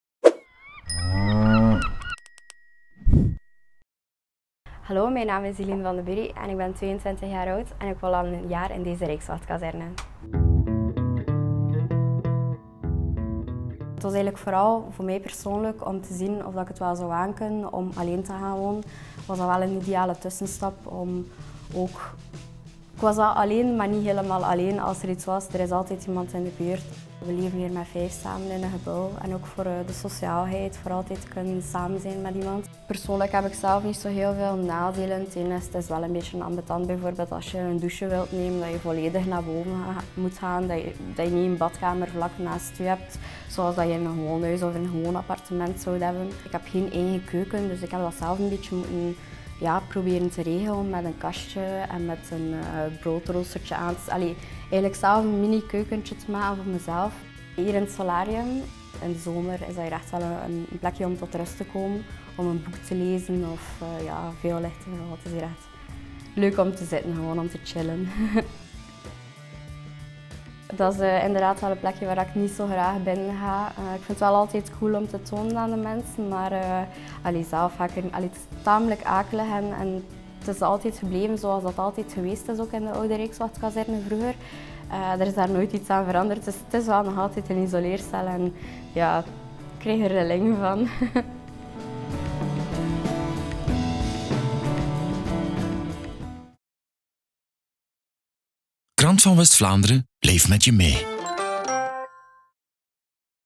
Antikraak - Eline uit Roeselare
Dit is het verhaal van Eline, een antikraker. 29 gebouwen van de federale overheid staan leeg in West-Vlaanderen. Om die panden tijdelijk een nut te geven, laat de overheid er mensen goedkoop in wonen: antikrakers. Zij beschermen het gebouw tegen krakers en verkrotting. Lees het dossier van Krant van West-Vlaanderen op